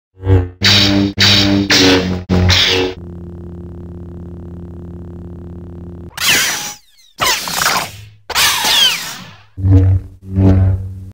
TheForce net Sound Effects Part 07: Lightsaber Sound Effects Part 2.
Here are some lightsaber sound effects part 2 that can be found on TheForce.net Internet.
20th Century Fox